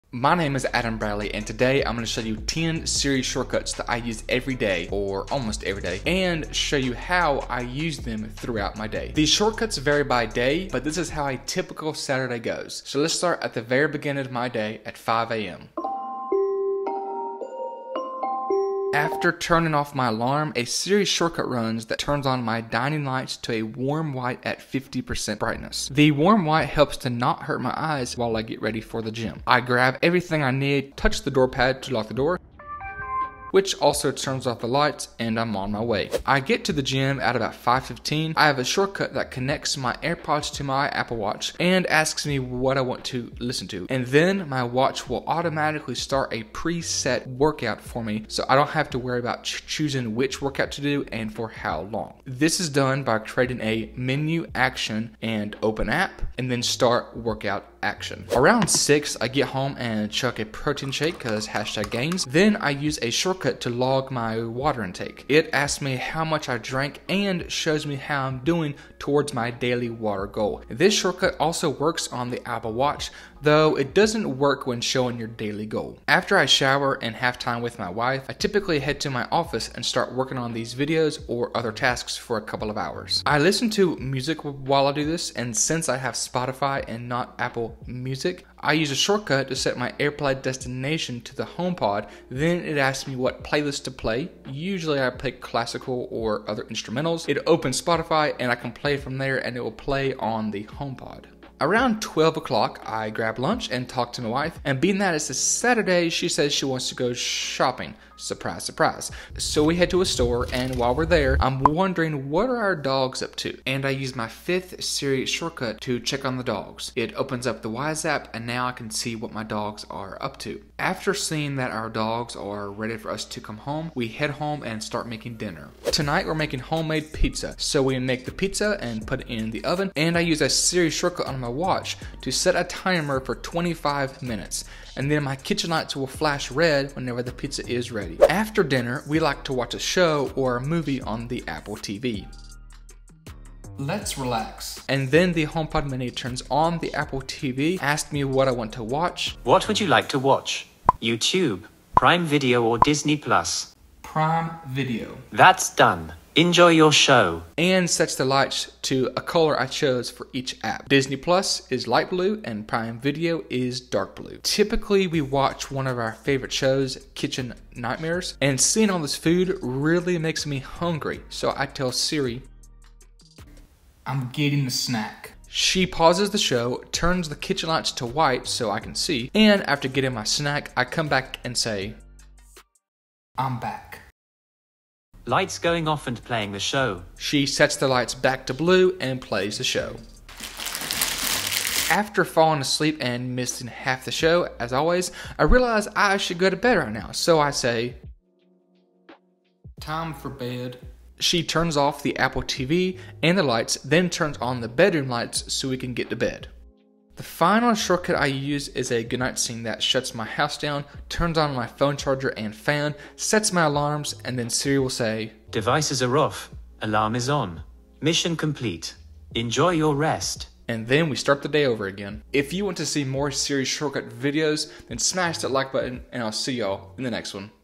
10 Siri Shortcuts I Use Everyday
I show you 10 Siri Shortcuts I use daily and how I use them throughout my day. I love using Siri Shortcuts and I wanted to share 10 that I think you may enjoy in your day as well.

⏰ CHAPTERS ⏰
0:00 - Intro
0:13 - Shortcut #1. Alarm Off, Kitchen Lights Auto On 50%, Warm White
0:37 - Shortcut #2. Start Workout Faster (Apple Watch)
0:59 - Shortcut #3. Log Water Intake
1:16 - Shortcut #4. Play Spotify on the HomePod
1:41 - Shortcut #5. Check on Dogs (Cameras)
2:01 - Shortcut #6. Pizza Timer (Apple Watch)
2:18 - Shortcut #7. Let’s Relax (Apple TV + HomePod mini)
2:45 - Shortcut #8. I’m Getting A Snack (Apple TV)
2:59 - Bonus: I’m Back
3:12 - Shortcut #9. Time For Bed
3:27 - Shortcut #10. Goodnight

1. Alarm Off, Lights on: (Built into iOS 14 so I can’t share it)
2. Workout
3. Log Water
4. AirPlay Studio Mini
5. Check The Dogs
6. Pizza Timer
7. Let’s Relax
8. I’m getting a snack
Bonus: I’m back
9. Time for bed
10. Goodnight